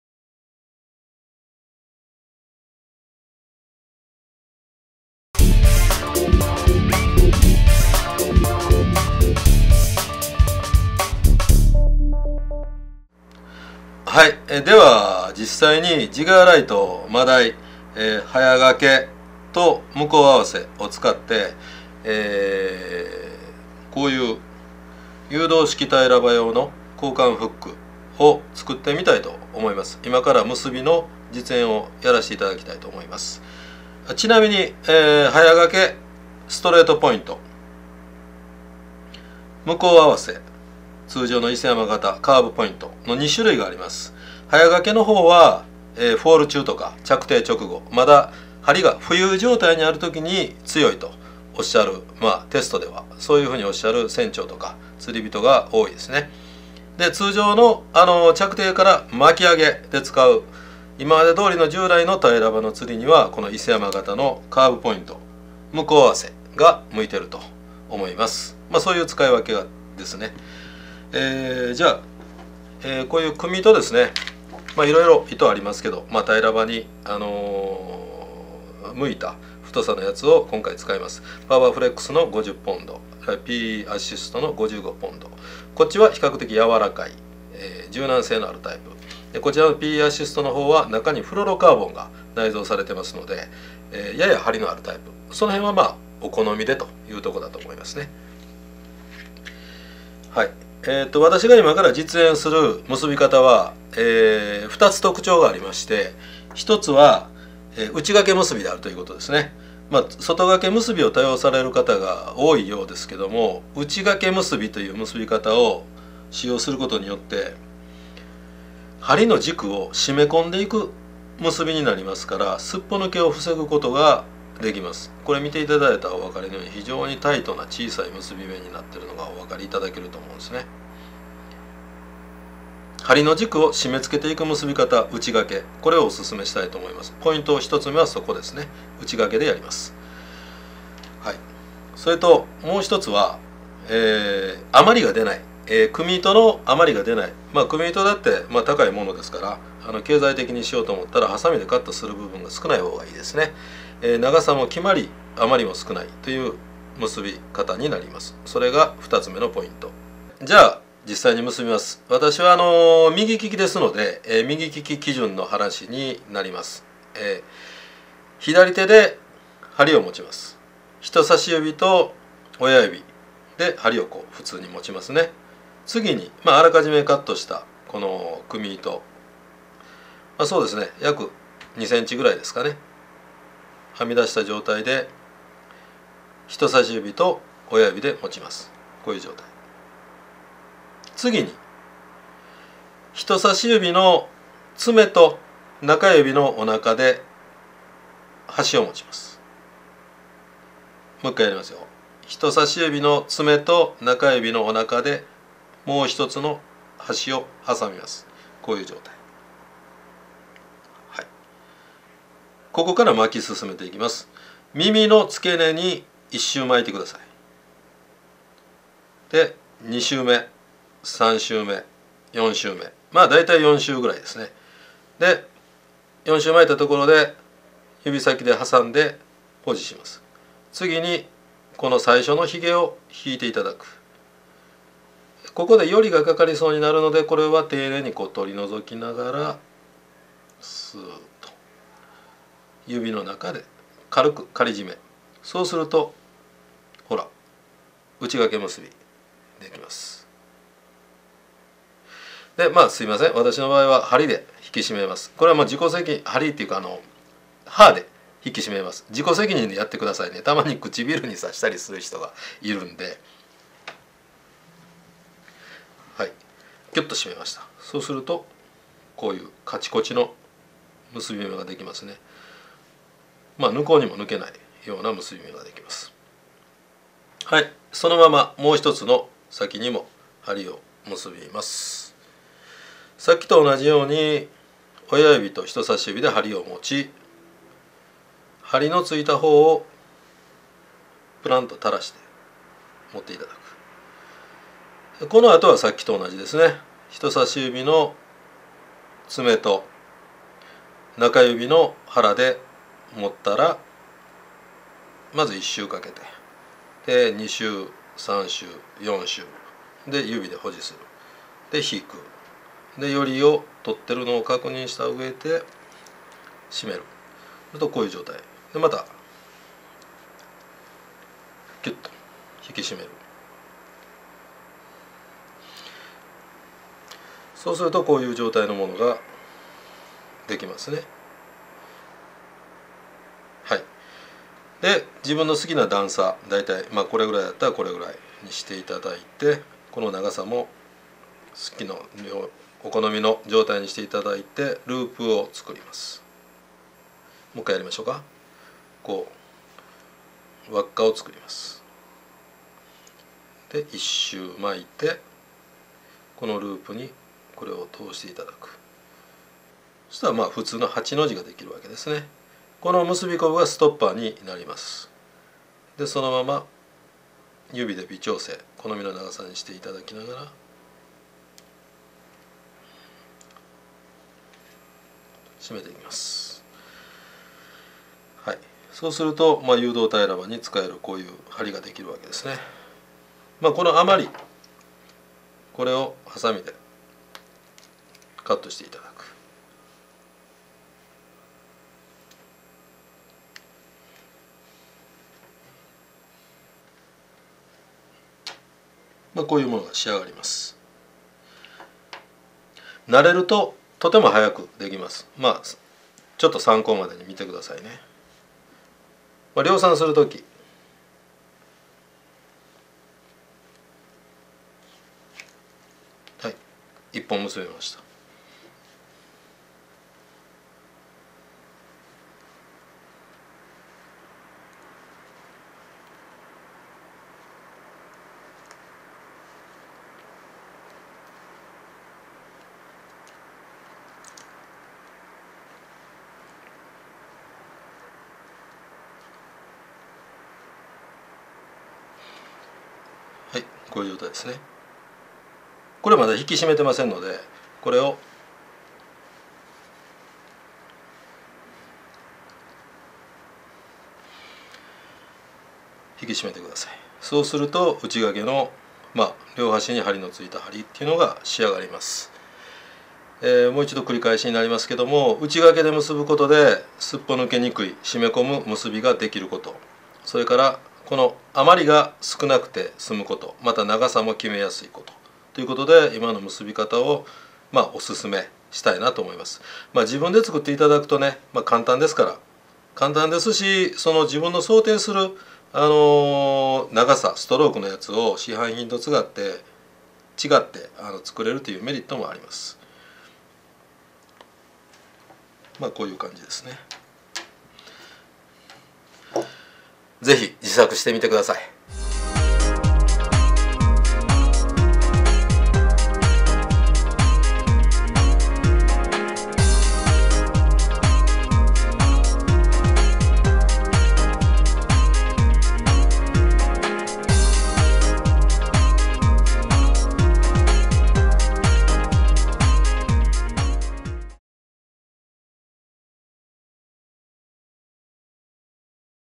タイラバ自作フックの結び方（ジガーライトマダイ使用）
今春発売のタイラバ専用フック「ジガーライトマダイ」を使ったタイラバフック作成動画です。
簡単に作成できますので、自作フックにチャレンジしてみてはいかがでしょうか。